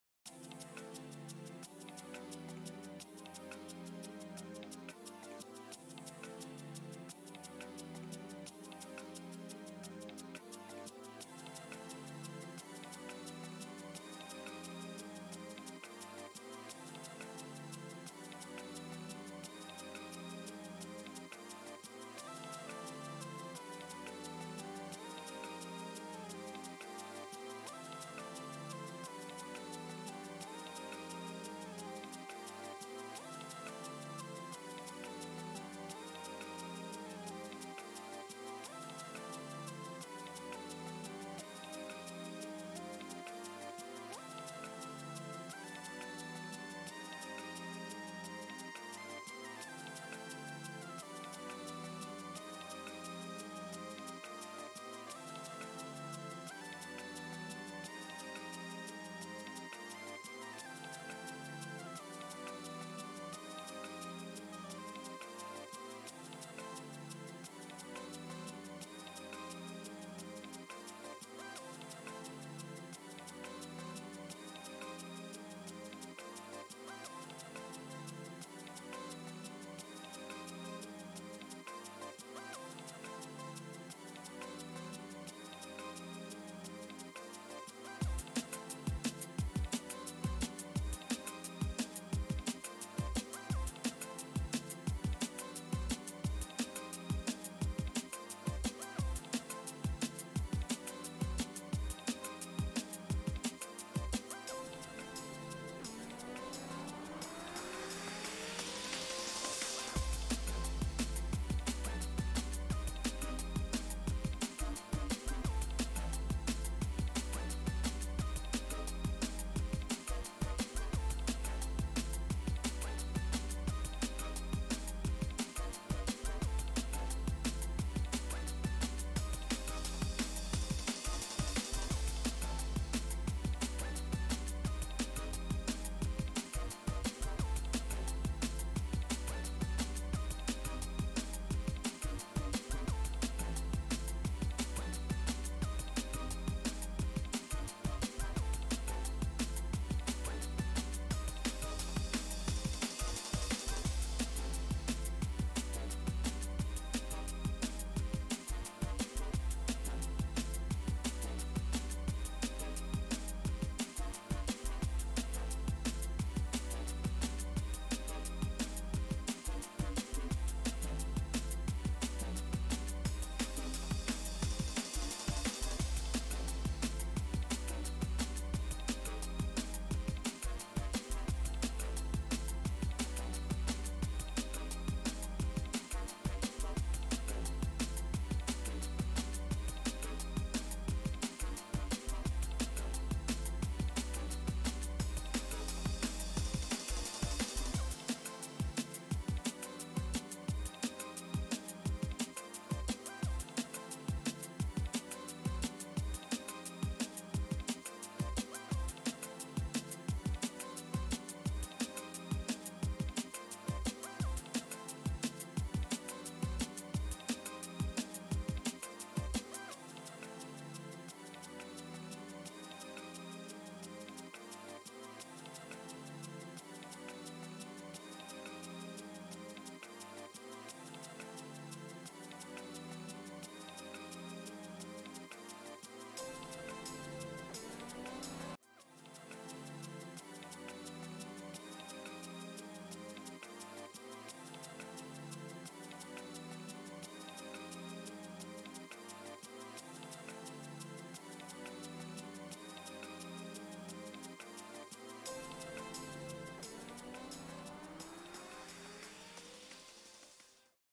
Monogrammed Nails - Monogram Manicure - Monogram Nail Design - Monogram Nail Art
If you like Monogrammed Nails this is the right manicure for you. It's a super cute way to customize your nails. Using this method create a unique nail look with your chosen nail polish colours. This classic method transforms an ordinary manicure into a unique masterpiece.